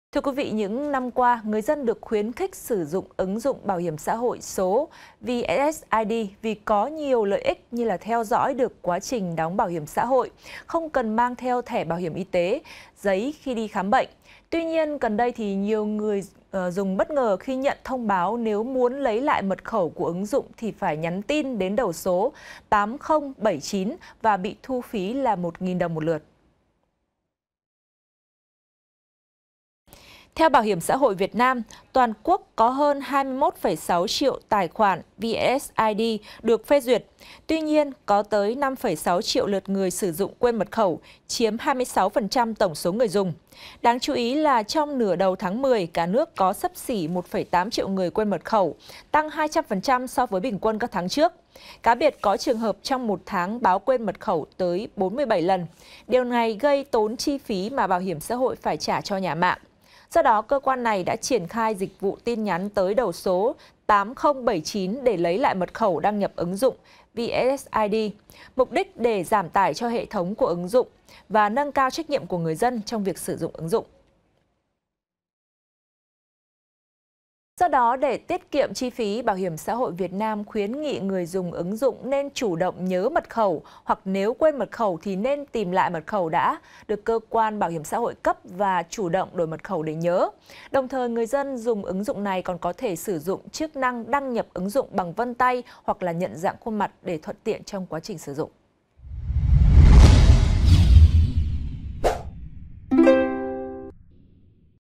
Tranh cãi việc lấy mật khẩu ứng dụng BHXH phải trả 1000 đồng | VTC14
VTC14 | TRANH CÃI VIỆC LẤY MẬT KHẨU ỨNG DỤNG BHXH PHẢI TRẢ 1000 ĐỒNG
Những năm qua, người dân được khuyến khích sử dụng ứng dụng BHXH số - VssID vì có nhiều lợi ích như theo dõi được quá trình đóng BHXH, không cần mang thẻ BHYT giấy khi đi khám bênh… Tuy nhiên, gần đây, nhiều người dùng bất ngờ khi nhận thông báo nếu muốn lấy lại mật khẩu của ứng dụng thì phải nhắn tin đến đầu số 8079 và bị thu phí 1000 đồng/lượt.